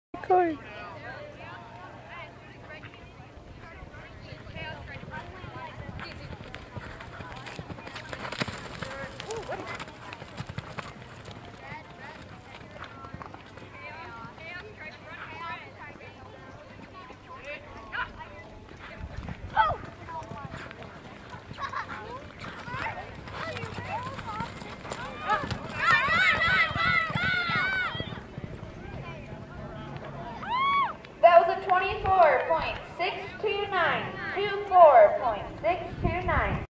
Gymkhana - April 27 - Good Friday on poles
Friday's 2nd time running poles. He ran a 24.6 and got first place! He's smoothing out quite a bit. Awesome camera angle too!